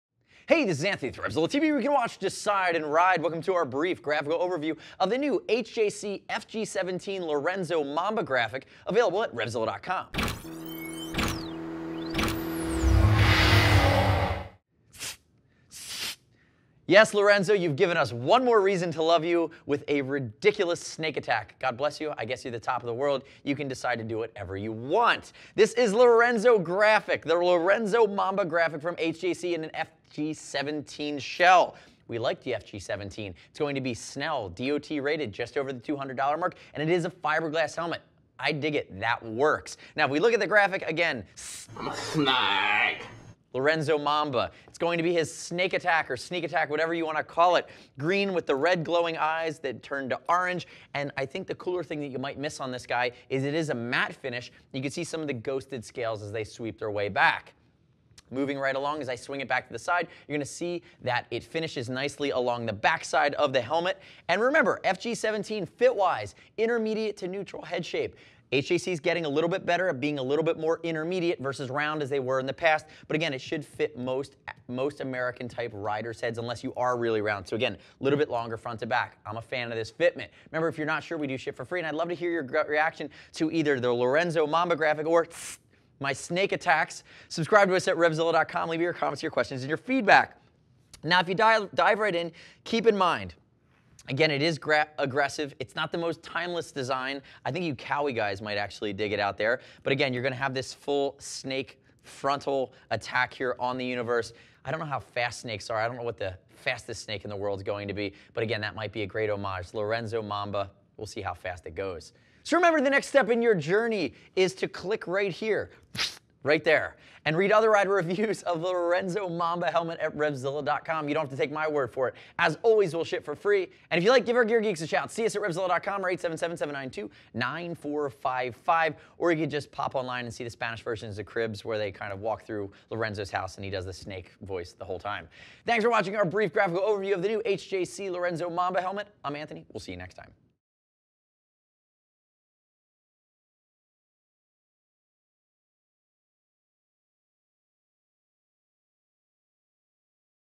HJC FG 17 Lorenzo Mamba Helmet Review at RevZilla.com
HJC FG 17 Lorenzo Mamba Helmet Review Are you a budding herpetologist with a penchant for MotoGP?  Yes?  Well, HJC must have read your mind.  The FG-17 Mamba (you can thank Lorenzo) comes complete with scales, snake eyes, and a venom pouch in the chin bar.  Okay, so there's no venom, but your competition doesn't know that.  They won't see you coming with your matte paint job either, so you can hunt your prey and steal pole position without a hitch.  Or for you dangerous types, the bright green colorway will sow terror in the paddock.